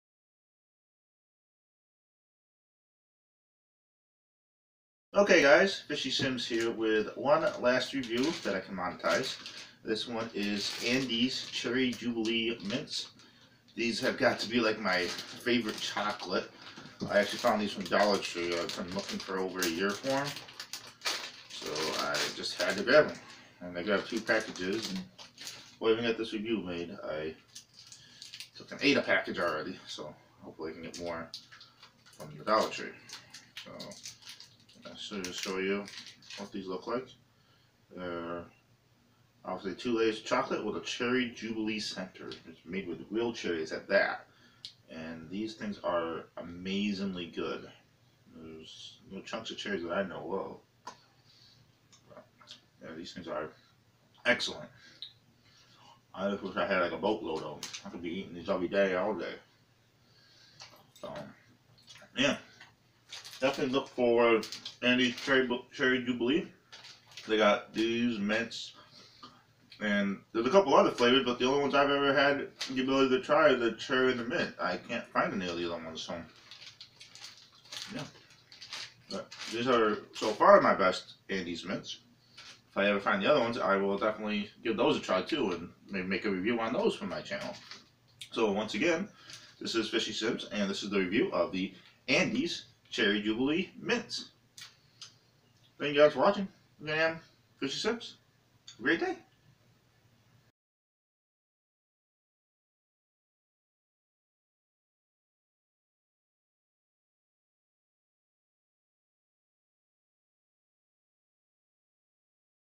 Andes Cherry Jubilee Thins - Review
Just my review of Andes Amazing Chocolate thins.  I would love to try and review all flavors once I get my hands onto them. So far Andes Cherry Jubilee is my favorite chocolate.  Their Mint flavor would be Second.